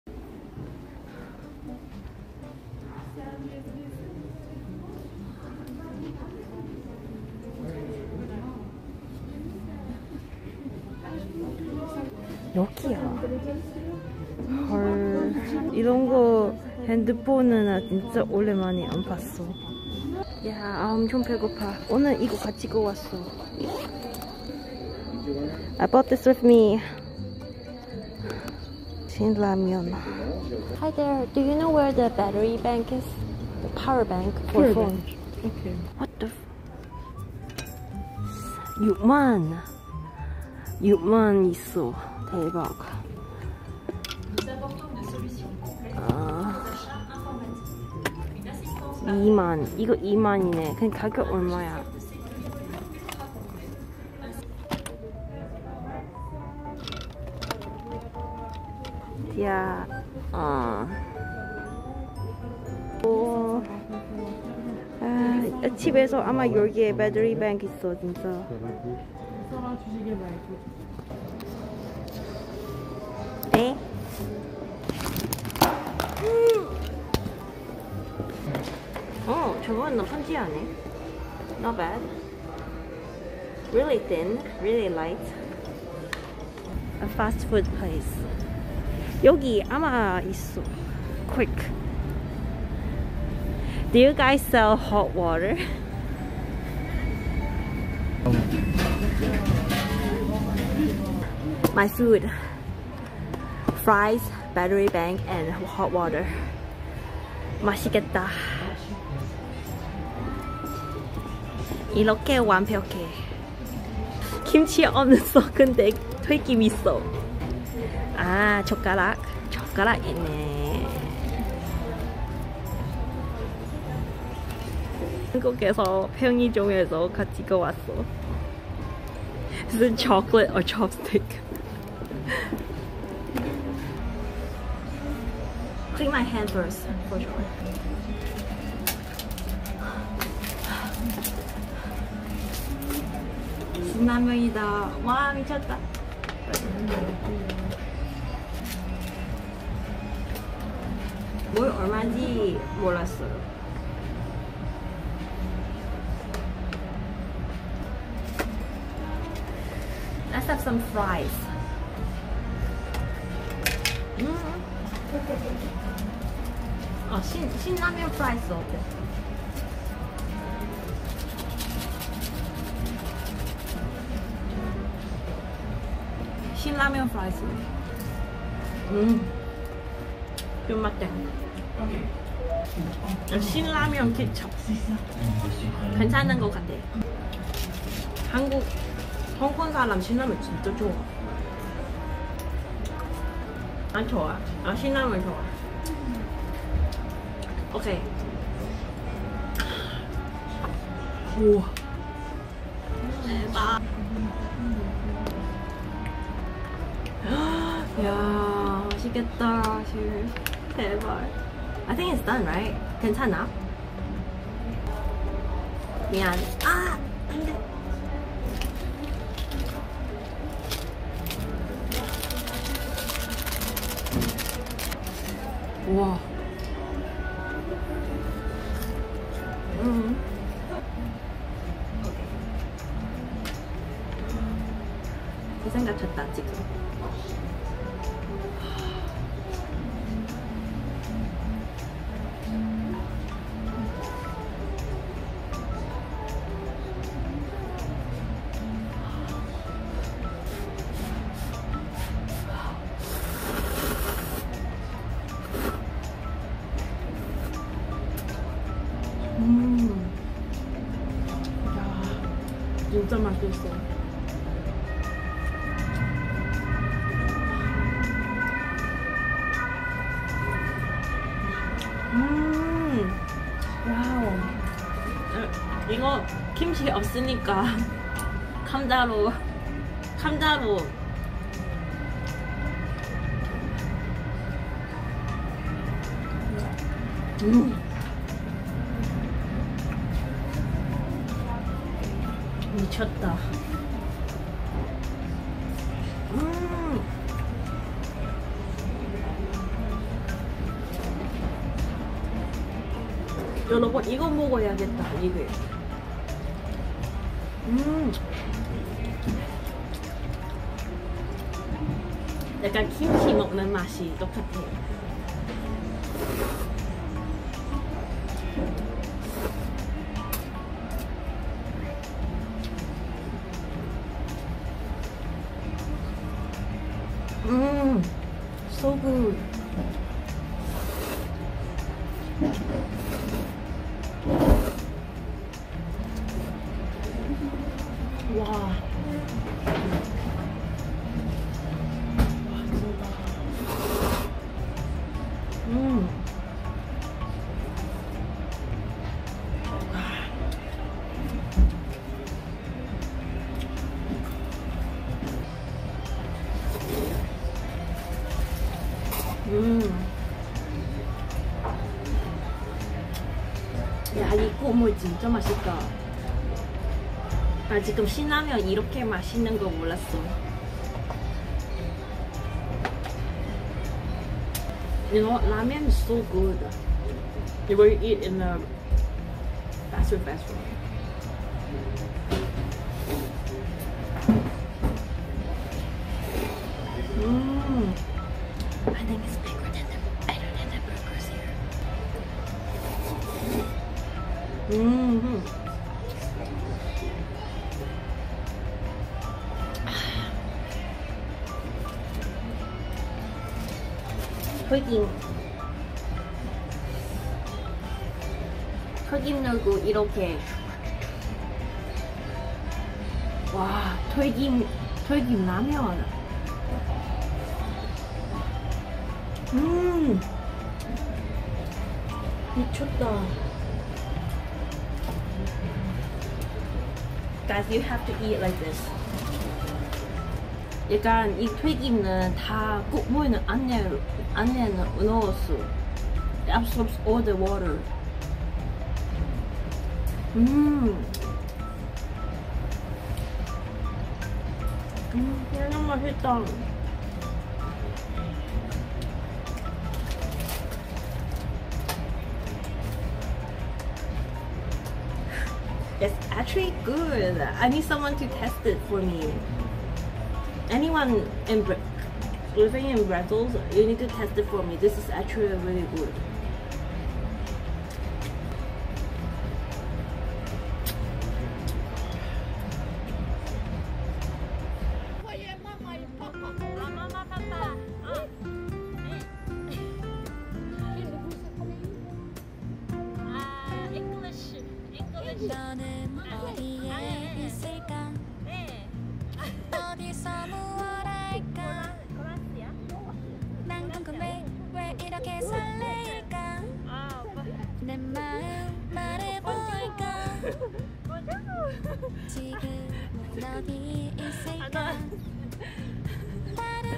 Secretly eating ramen at a fast food restaurant in Belgium
Music from BGM presidents